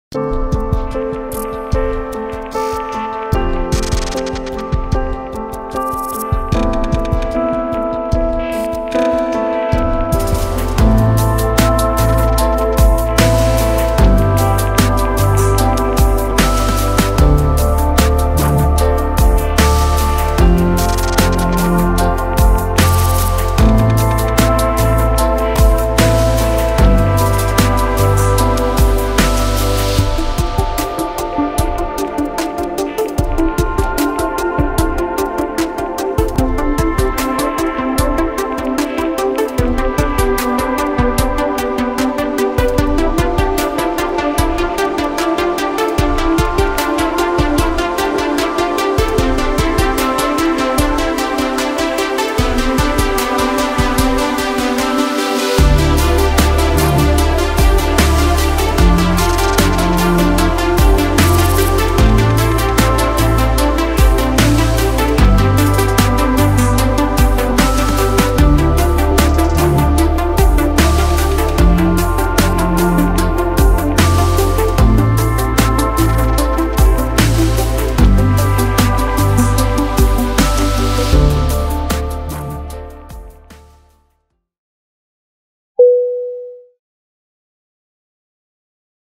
Firefly Aircraft Cleaning
Firefly conducts regular deep cleaning of the aircraft for the safety of all our passengers. Watch this video to see how we do it 😊